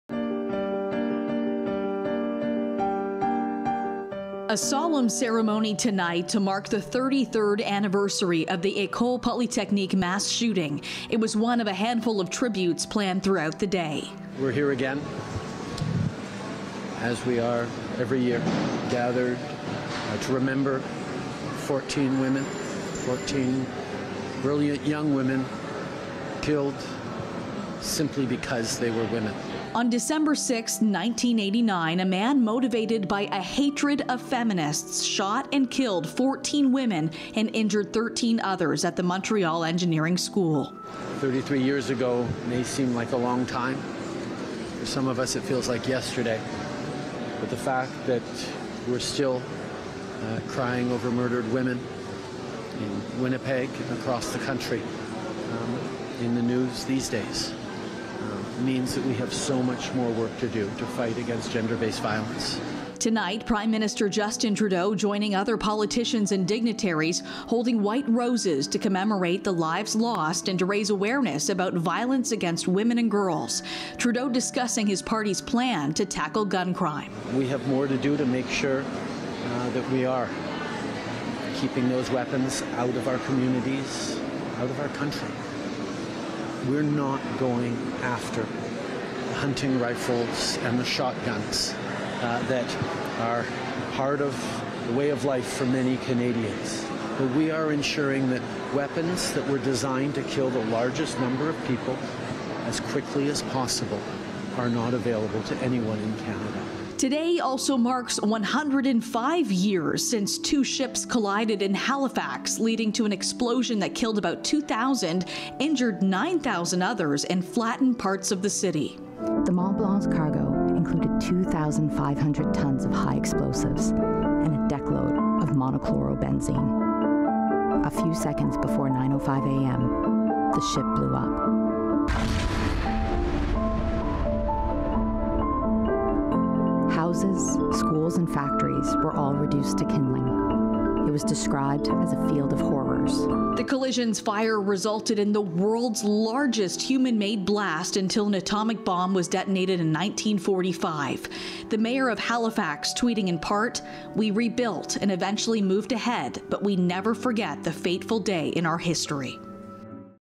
Polytechnique mass shooting and Halifax explosion being remembered online and across Canada today
December 6th marks a devastating day as we look back at the Montreal massacre and an explosion in Halifax more than 100 years ago that wiped out the city and killed thousands.